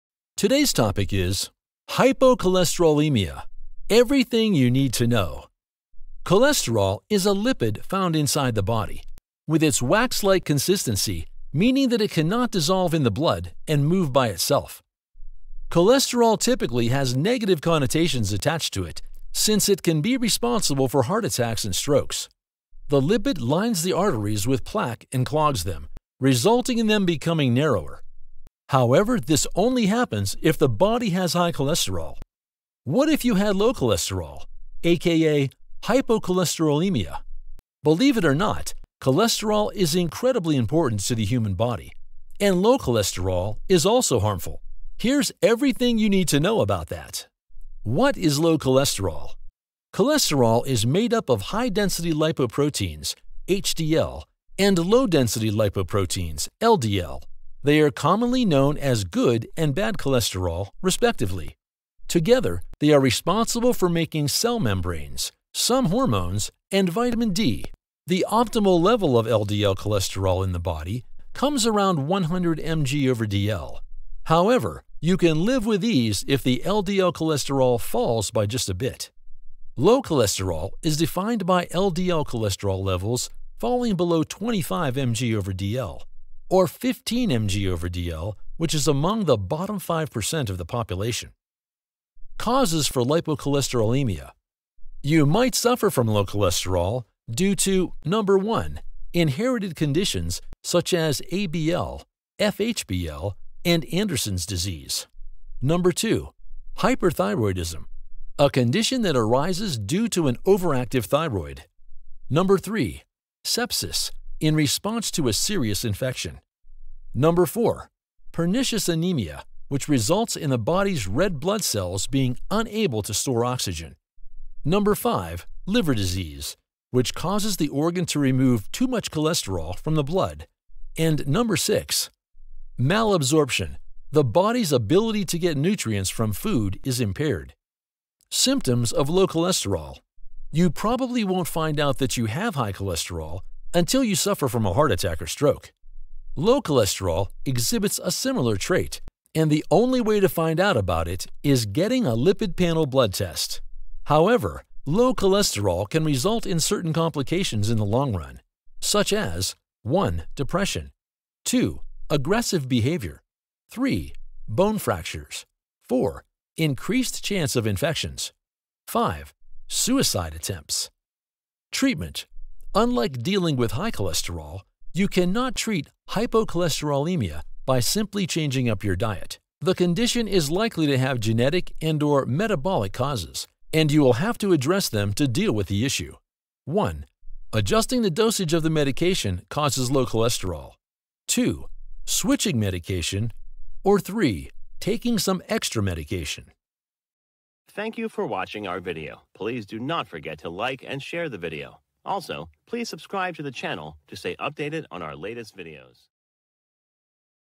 Hypocholesterolemia: Everything You Need to Know
Chapters

0:00 Introduction
0:33 What if you had low cholesterol, aka hypocholesterolaemia?
0:49 what is low cholesterol?
1:33 Causes of Hypocholesterolaemia
2:18 Symptoms of low cholesterol
2:52 Treatment of Hypocholesterolemia

Hypocholesterolemia is the presence of abnormally low (hypo-) levels of cholesterol in the blood (-emia). A defect in the body's production of cholesterol can lead to adverse consequences as well. Cholesterol is an essential component of mammalian cell membranes and is required to establish proper membrane permeability and fluidity. It is not clear if a lower than average cholesterol level is directly harmful; however, it is often encountered in particular illnesses.With the increased use of medication to suppress cholesterol, some have expressed concern that lowering cholesterol levels excessively will itself cause disease.
Specific disease entities

Demographic studies suggest that cholesterol levels form a U-shape curve when plotted against mortality; this suggests that low cholesterol is associated with increased mortality, mainly due to depression, cancer, hemorrhagic stroke, aortic dissection and respiratory diseases.[1] It is possible that whatever causes the low cholesterol level also causes mortality, and that the low cholesterol is simply a marker of poor health.[2]

Links with depression have been supported by studies.[3] In contrast, no evidence was found for a link with hemorrhagic stroke (although higher cholesterol levels conferred a relative protection), and neither did statin drugs worsen the risk.[4]

The Heart Protection Study found no increase in either respiratory disease or neuropsychiatric illness in a large trial population taking a statin drug.[5]
Elderly

In the elderly, low cholesterol may confer a health risk that may not be offset by the beneficial effects of cholesterol lowering.[6] Similarly, for elderly patients admitted to hospital, low cholesterol may predict short-term mortality.[7] The prevalence of hypocholesterolemia in the elderly ranges between 2% to 36%, depending on specific cutoff levels and age range investigated.[8] Alerting physicians to hypocholesterolemia may benefit some of their patients who take cholesterol-lowering drugs and decrease the rate of their emergency room visits.[9]
Critical illness

In the setting of critical illness, low cholesterol levels are predictive of clinical deterioration, and are correlated with altered cytokine levels.[10]